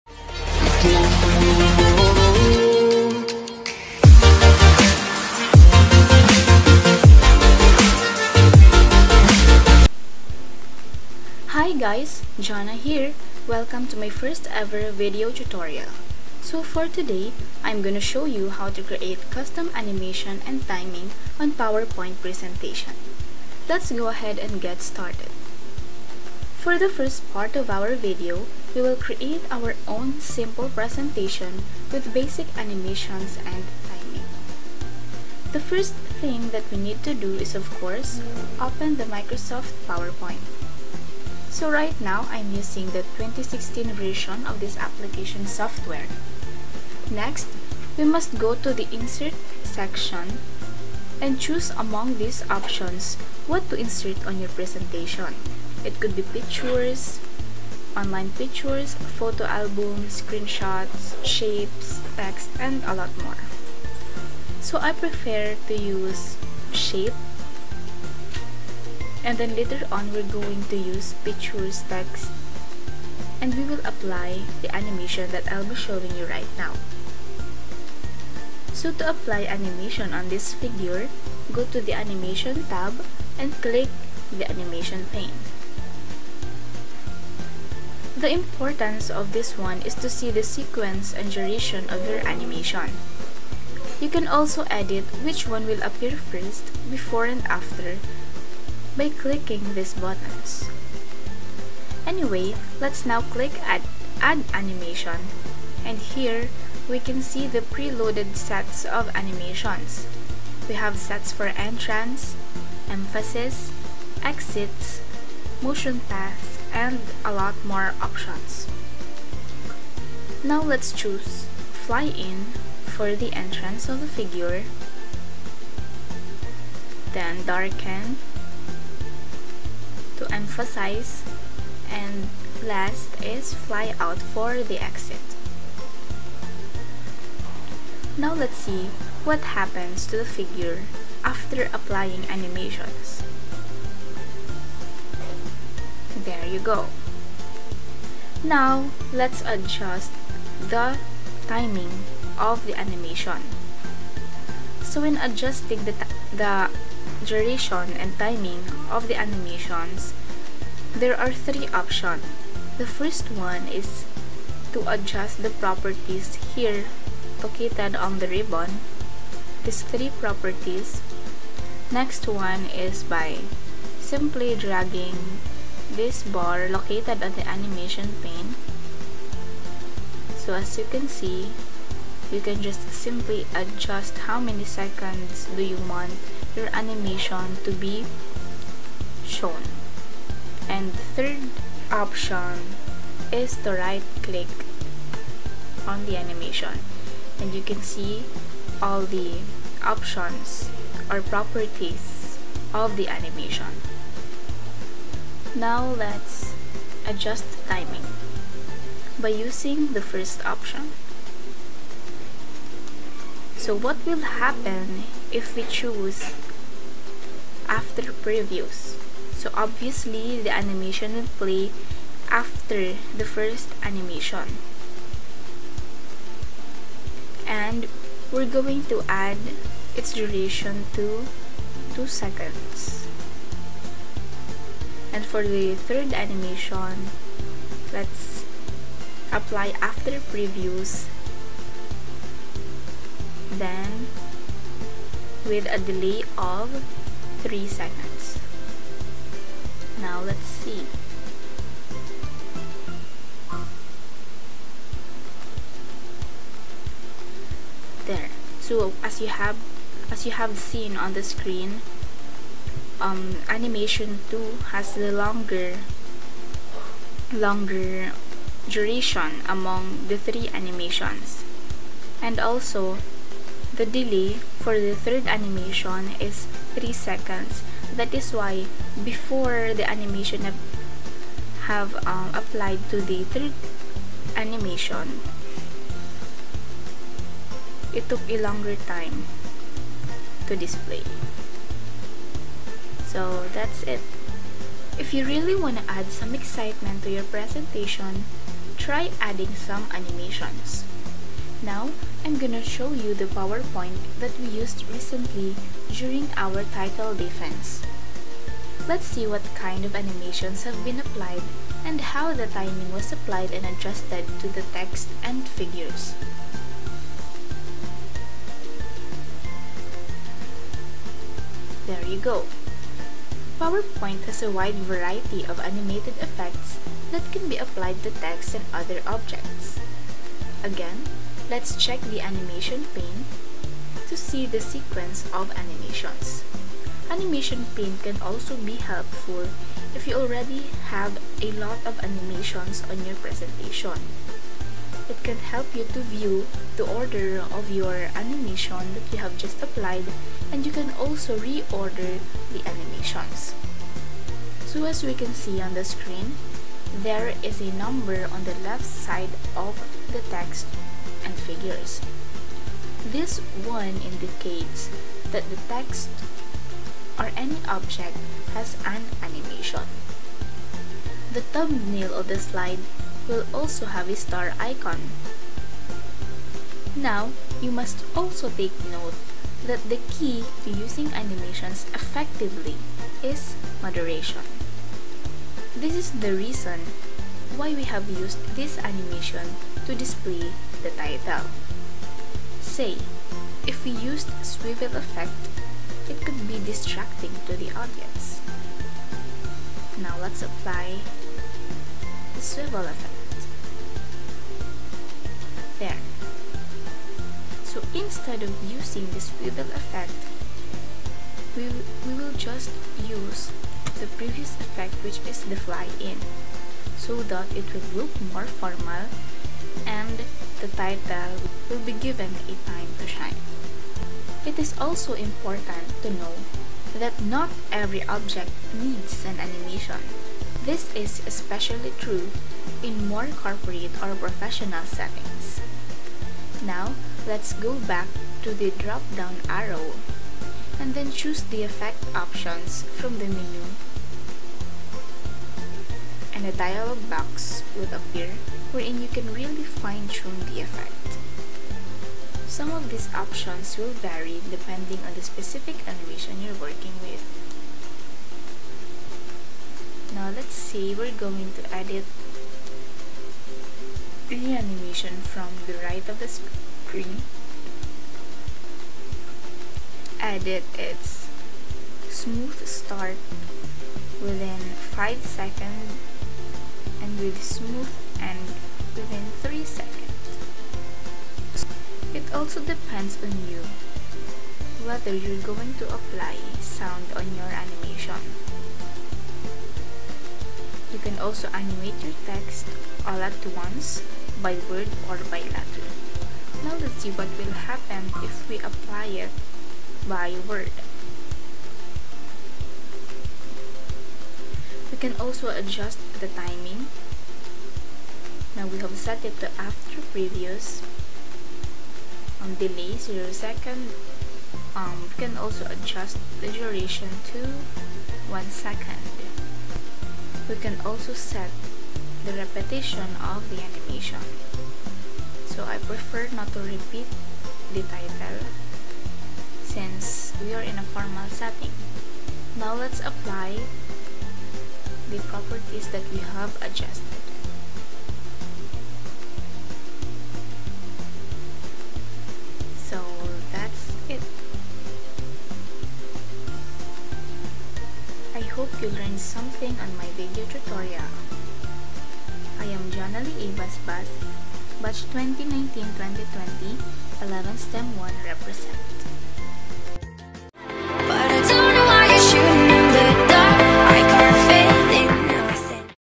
Custom Animation and Timing on Powerpoint Presentaions
Hello everyone! In this video, you're going to learn how to apply animations on powerpoint presentations in different ways, and how to adjust the timing. You will also learn the importance of animations, timing, and other elements related to them. (You can also turn on the subtitle of this video to understand more what I was talking about. Kindly excuse the quality of my audio and my voice as well haha!)